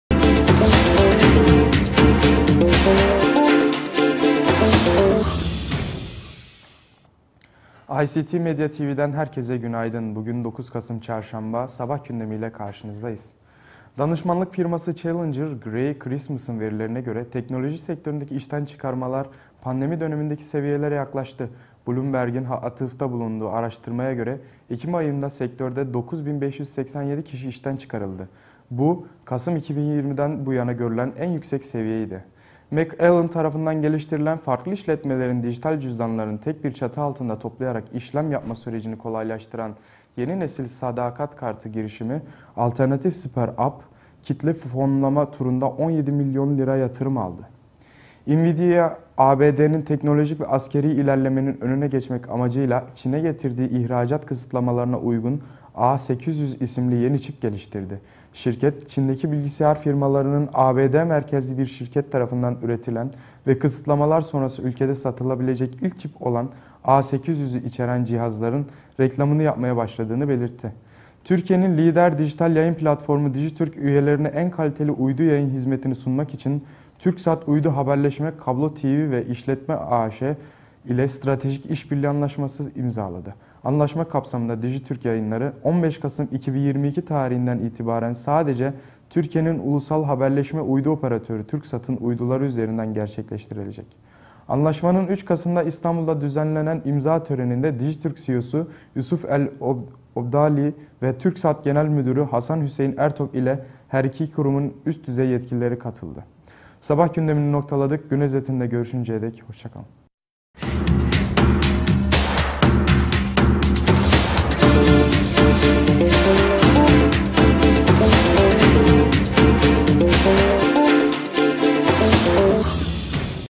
Digiturk Ve TÜRKSAT Stratejik İş Birliği Anlaşması İmzaladı
Danışmanlık firması Challenger, Gray & Christmas'ın verilerine göre, teknoloji sektöründeki işten çıkarmalar, pandemi dönemindeki seviyelere yaklaştı. Bloomberg'ün atıfta bulunduğu araştırmaya göre, ekim ayında sektörde 9 bin 587 kişi işten çıkarıldı. Bu, Kasım 2020'den bu yana görülen en yüksek seviyeydi.

Macellan tarafından geliştirilen, farklı işletmelerin dijital cüzdanlarını tek bir çatı altında toplayarak işlem yapma sürecini kolaylaştıran yeni nesil sadakat kartı girişimi Alternatif SuperApp, kitle fonlama turunda 17 milyon lira yatırım aldı.

Nvidia, ABD'nin teknolojik ve askerî ilerlemenin önüne geçmek amacıyla Çin'e getirdiği ihracat kısıtlamalarına uygun "A800" isimli yeni çip geliştirdi. Şirket, Çin'deki bilgisayar firmalarının, ABD merkezli bir şirket tarafından üretilen ve kısıtlamalar sonrası ülkede satılabilecek ilk çip olan A800'ü içeren cihazların reklamını yapmaya başladığını belirtti.

Anlaşma kapsamında Digiturk yayınları, 15 Kasım 2022 tarihinden itibaren sadece, Türkiye'nin ulusal haberleşme uydu operatörü TÜRKSAT’ın uyduları üzerinden gerçekleştirilecek. Anlaşmanın 3 Kasım’da İstanbul’da düzenlenen imza törenine Digiturk CEO’su Yousef Al-Obaidly ve TÜRKSAT Genel Müdürü Hasan Hüseyin Ertok ile her iki kurumun üst düzey yetkilileri katıldı.